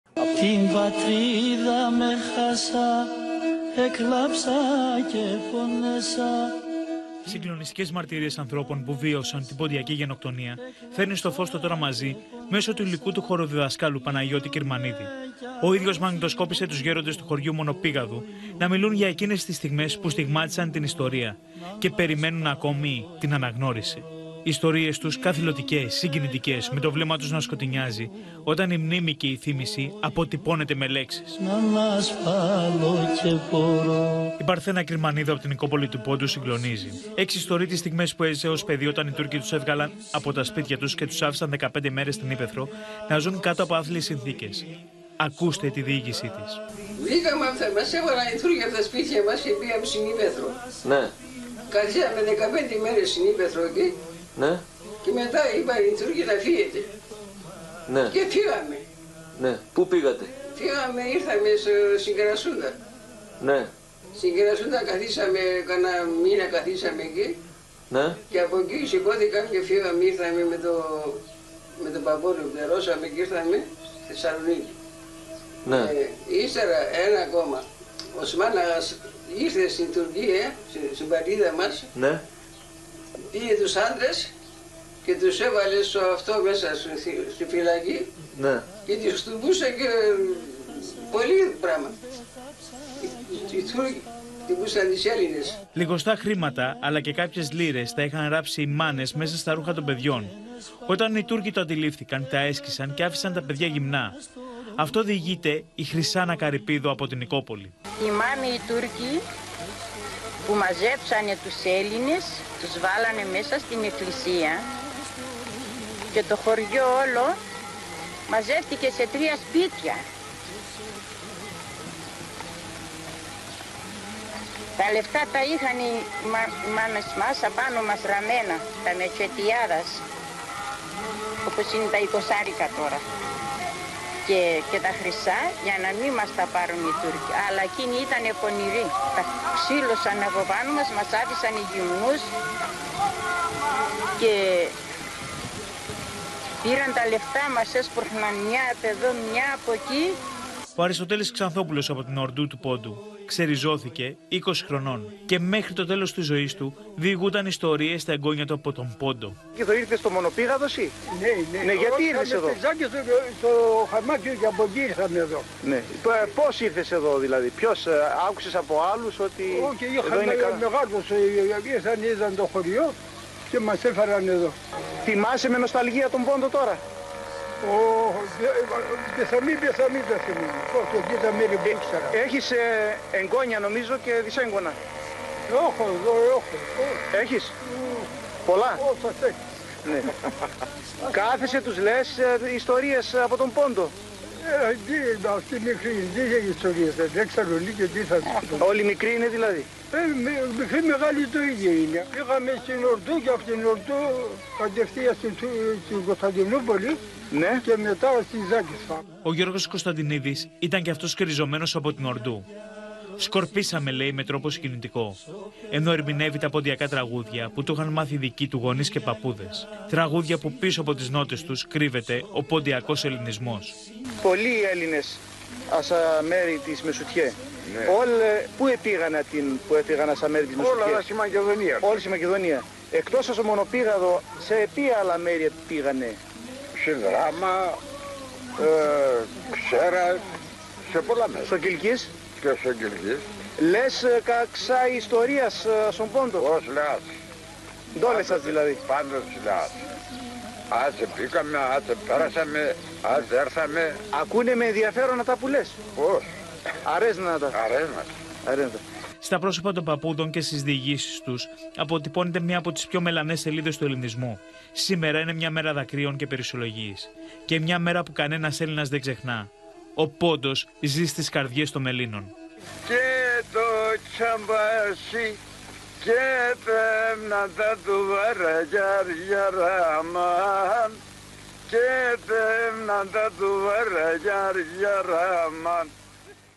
Ντοκουμέντο: Συγκλονιστικές μαρτυρίες ανθρώπων που βίωσαν την ποντιακή γενοκτονία | OPEN TV
Σε ένα συγκλονιστικό βίντεο άνθρωποι που βίωσαν την ποντιακή γενοκτονία, μιλούν για όλα όσα έζησαν αυτές τις φρικτές μέρες.